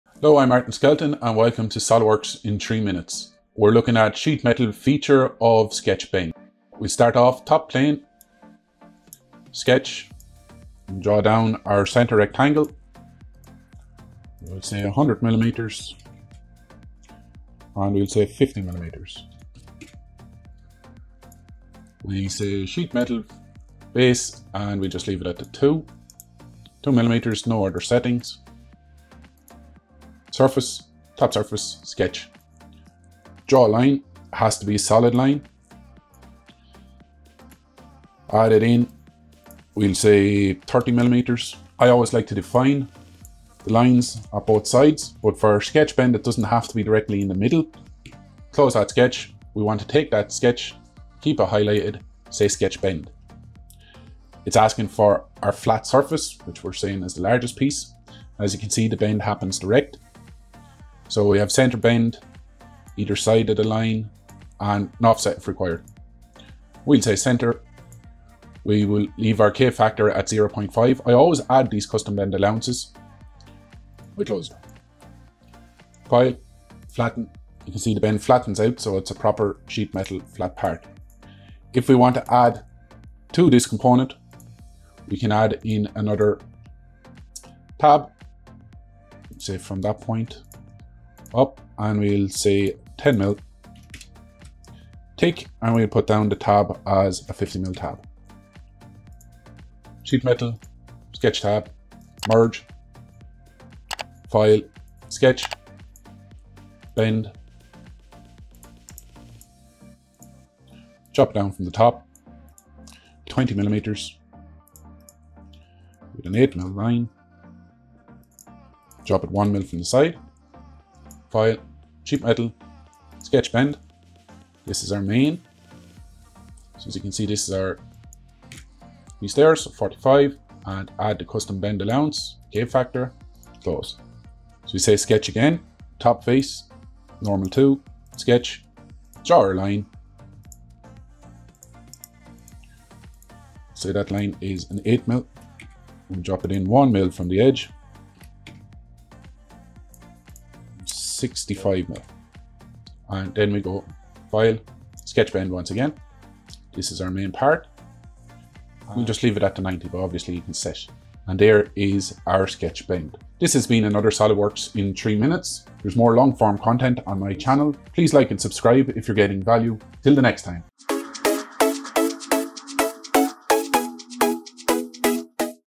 SOLIDWORKS FEATURE IN 3 MINUTES // SKETCH BEND
Two different methods of using the SKETCH BEND feature in Solidworks Sheet Metal. Method (1) Full Plate Bend. Method (2) Double Tab Bend. Solidworks 2022 Edge Flange feature.

This tutorial will work for different versions of solidworks 2021 etc and this tutorial series also shows some solidworks tips and tricks . The object of the tutorials is to teach the basics to computer-aided engineering that is used in the fabrication sector with machines such as CNC equipment. This basic learning is the production of DXF files that is used in 2D machining and can be opened with programs such as draftsight
This tutorial will work in:

This is reference tutorial 3 of a set for beginners in Solidworks sheet metal.

00:00 Intro / Layout
00:30 Method 1
01:30 Method 2